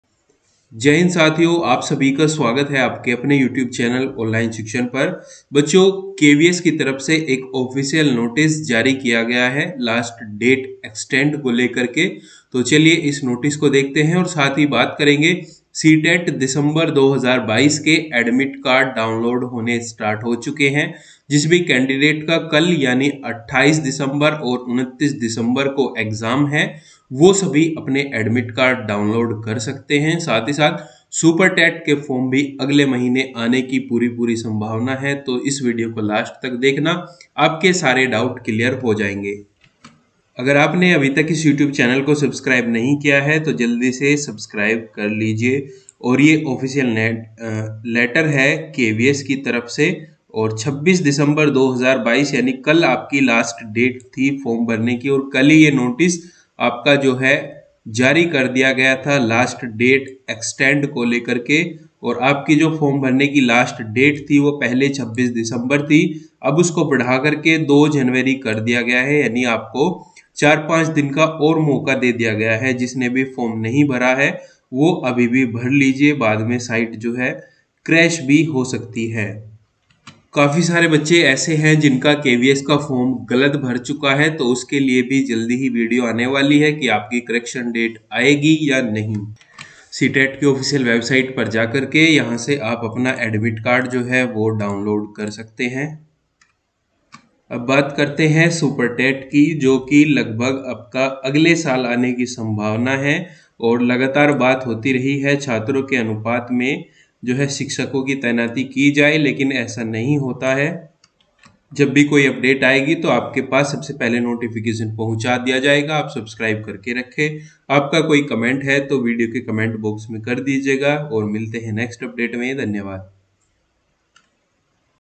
जरूरी सूचना | KVS अंतिम तिथि बढ़ा दी गई | CTET Admit card download | KVS last date extended
Hello Friends,

दोस्तों PDF के लिए टेलीग्राम पर भी हमें ज्वाइन करिए |
Note- अगर आप टेलीग्राम ग्रुप जॉइन नहीं कर पा रहे हैं तो आप टेलीग्राम पर जाकर online shikshan  सर्च कर सकते हैं